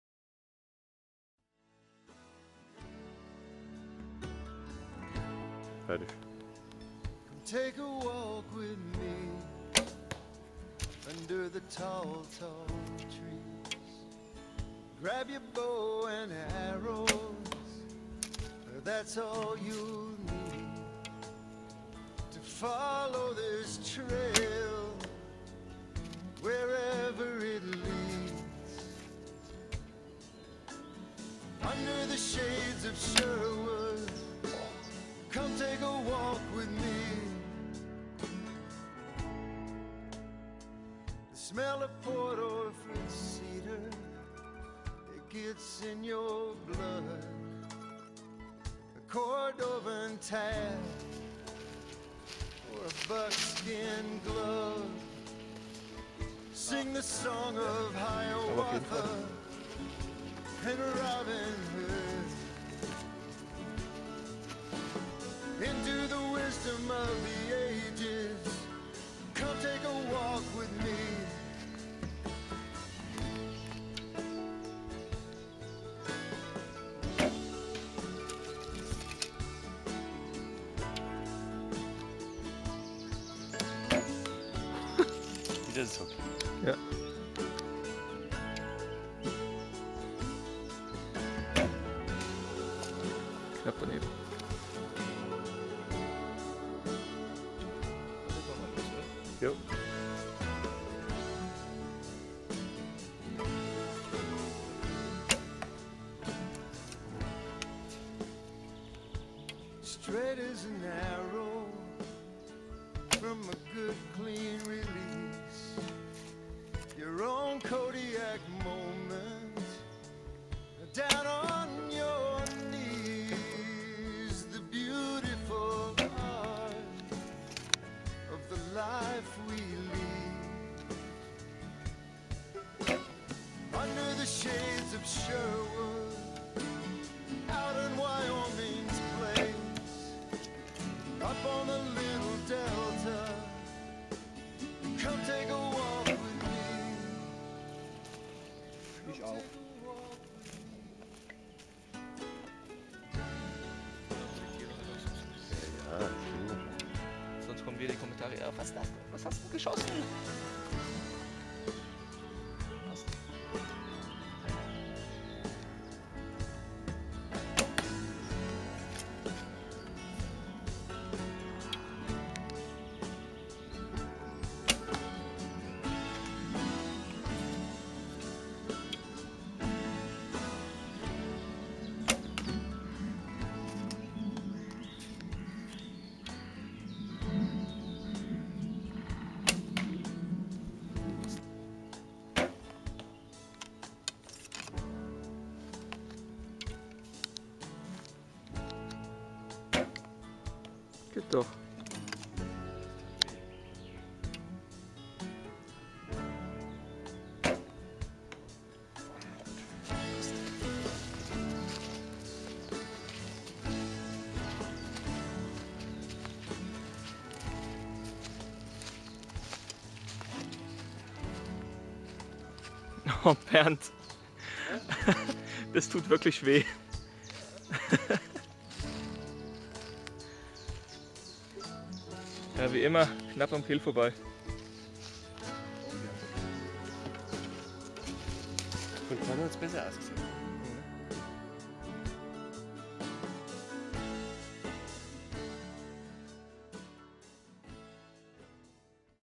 Archery at the Local 3D Range
Bernd and myself headed out to Eschau 3D range for a nice practice session

I do not claim rights or credits to this song